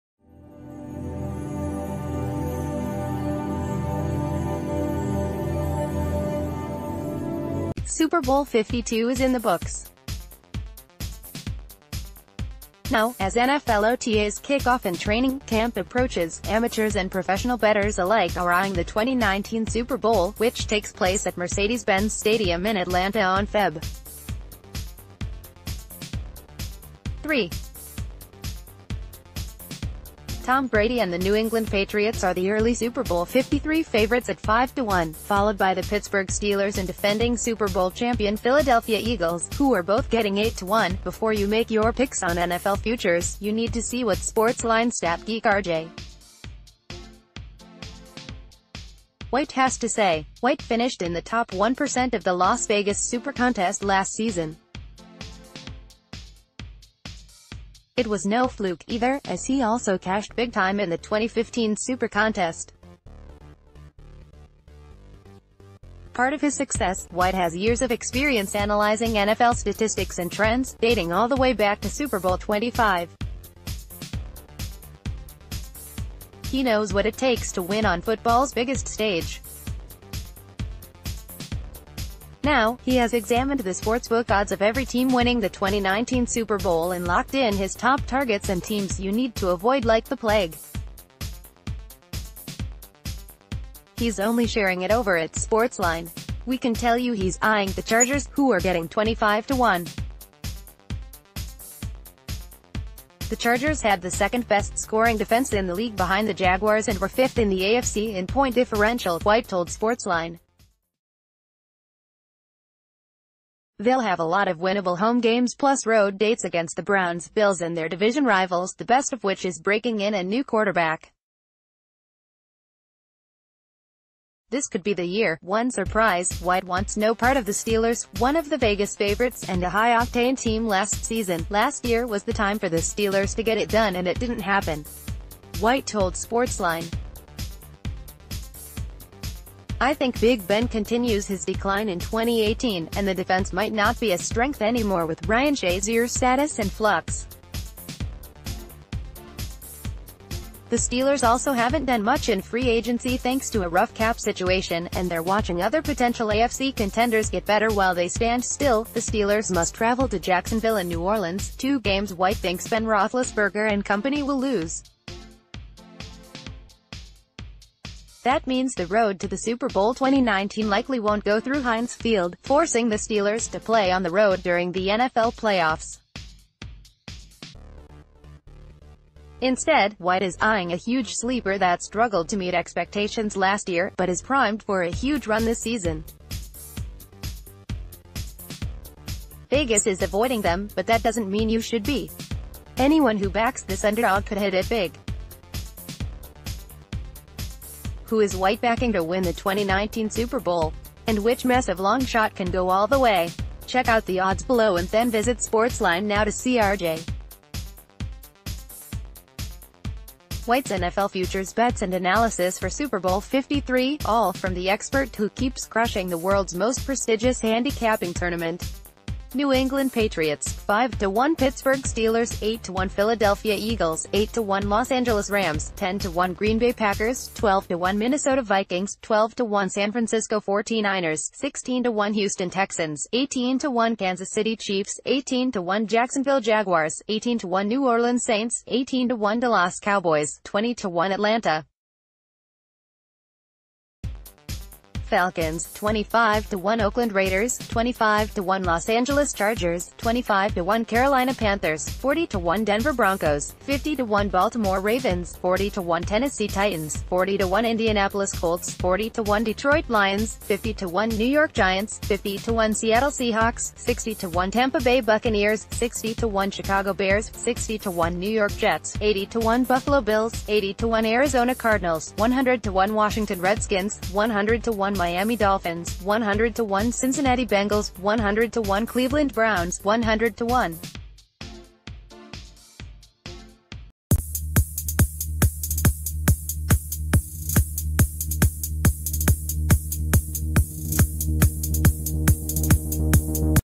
Breaking News | 2019 NFL Super Bowl odds: Vegas expert picks, predictions, location, teams to fade
Super Bowl LII is in the books. Now, as NFL OTAs kick off and training camp approaches, amateurs and professional bettors alike are eyeing the 2019 Super Bowl, which takes place at Mercedes-Benz Stadium in Atlanta on Feb. 3. Tom Brady and the New England Patriots are the early Super Bowl 53 favorites at 5-1, followed by the Pittsburgh Steelers and defending Super Bowl champion Philadelphia Eagles, who are both getting 8-1.Before you make your picks on NFL futures, you need to see what SportsLine...